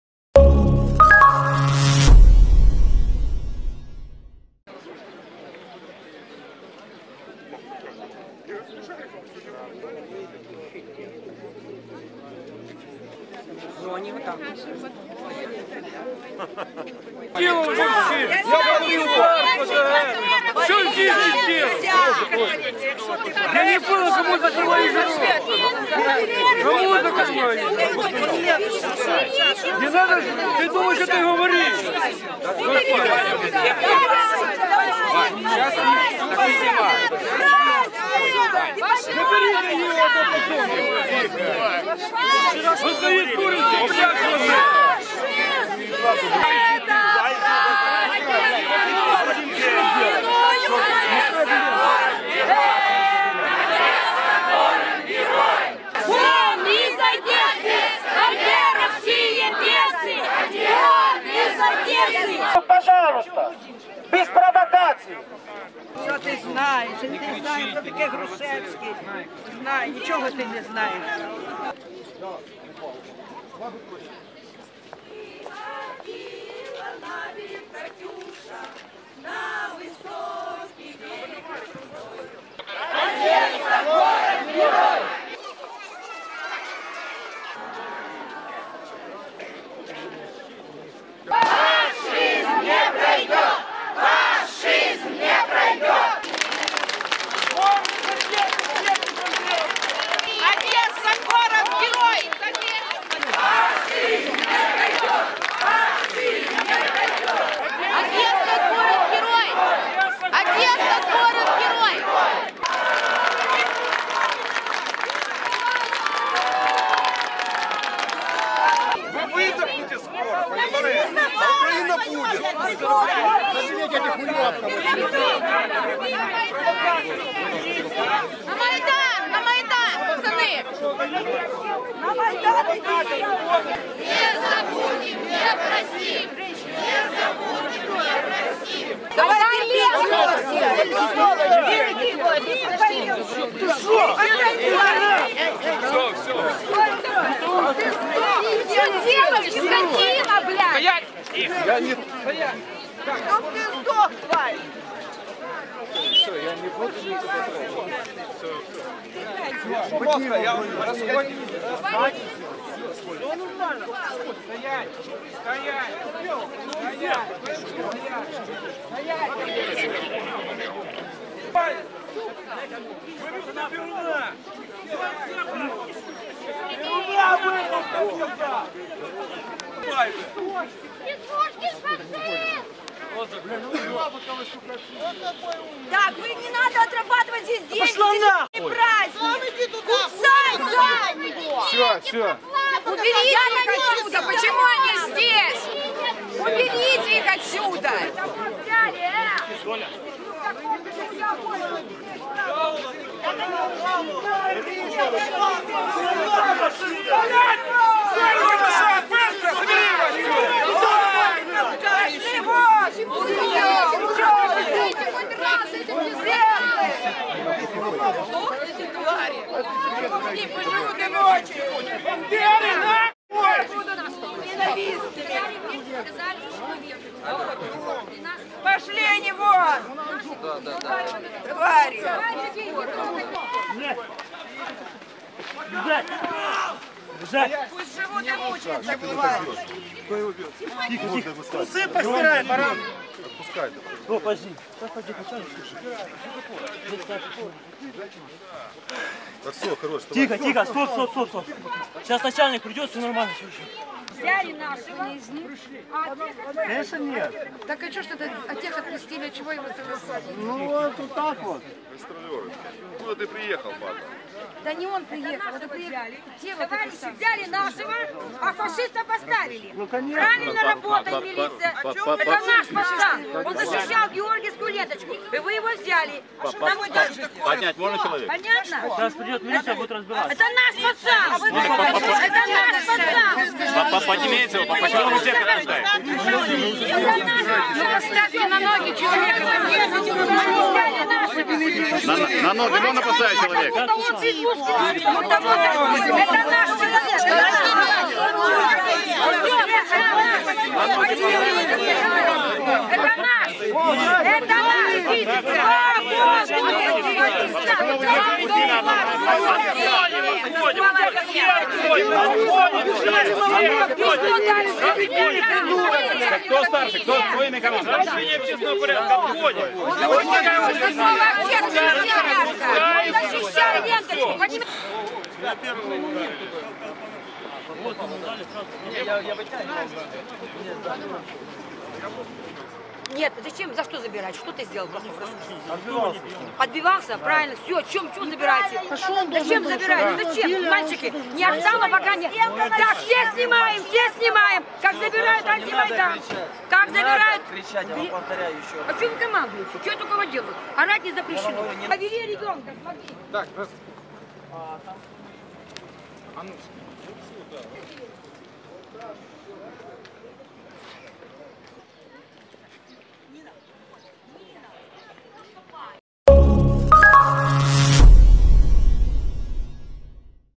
Бійка під час мітингу «Антимайдану» в Одесі на Алеї Слави 10.04.2016
Під час церемонії покладання квітів на Алеї Слави сталась бійка між активістами «Кулікового поля» та проукраїнськими активістами. Одного чоловіка звинуватили в тому, що він поцупив чорно-помаранчеву стрічку з одягу одного з митингувальників. Згодом правоохоронці затримали одного з учасників бійки. «Антимайданівці», які були присутні при цьому, вирішили, що затриманий - проукраїнський активіст. Вони почали його ганьбити та дякувати правоохоронцям. Але коли виявилося, що порушник навпаки, з їхнього оточення, миттєво змінили настрій на протилежний та вимагали звільнити затриманого.